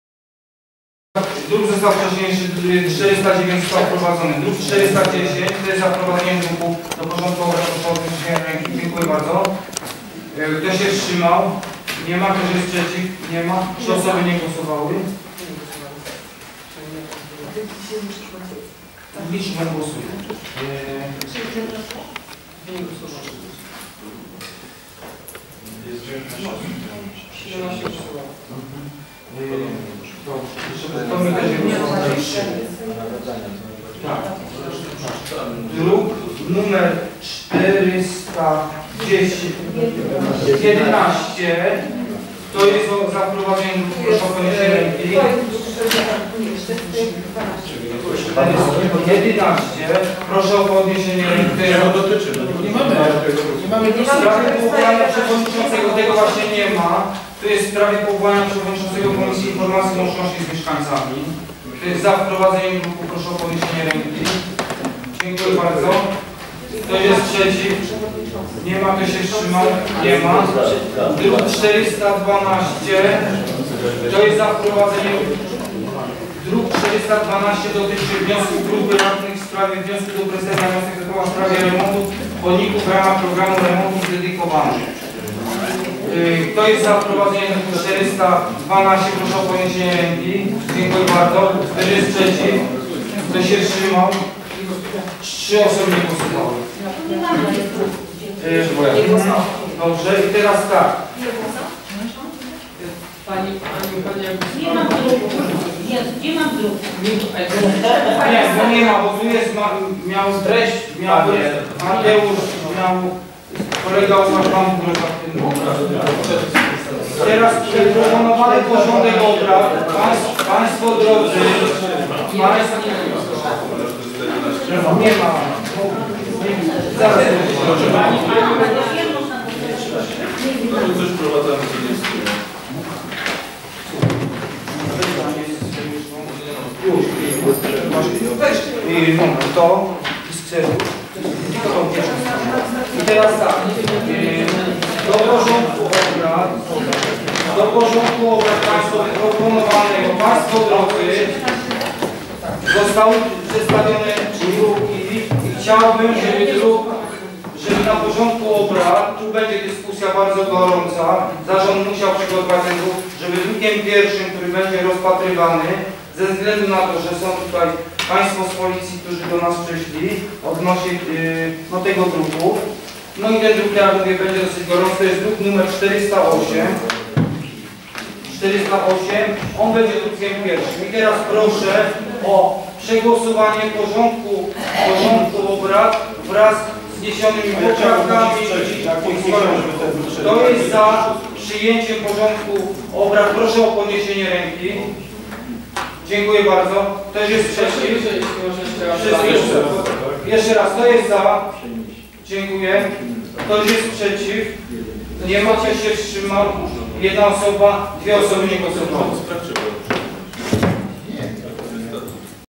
RD_III_28-05-2013_1 przyjęcie porządku obrad
Nagranie z XXXIII sesji Rady Dzielnicy III Prądnik Czerwony z dnia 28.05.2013 r.-cz.1- omawiane tematy i głosowania:

-przyjęcie porządku obrad